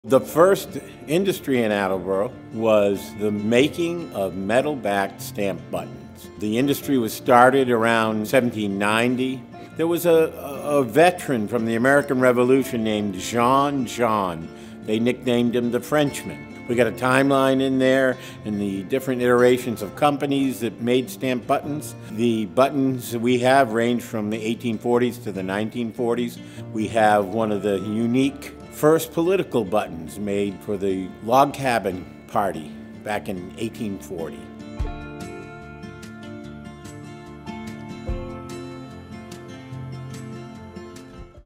180-Year Old Stamped Metal Buttons | Town, Presidential Buttons & More
Attleboro is home to some of the first-ever stamped metal buttons. In 1780, a man named “John John”—nicknamed “The Frenchmen”—started button making in the town, with many other successful businesses cropping up through the years.

At the bottom of the display case are presidential campaign buttons for William Henry Harrison—from more than 180 years ago. Buttons were also made for the military and city workers, which included firemen, the highway department, and even the sewer department. Most of the buttons you see here are from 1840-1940, but it also includes one of the oldest buttons known in the United States. When you come into the museum, we'll show it to you!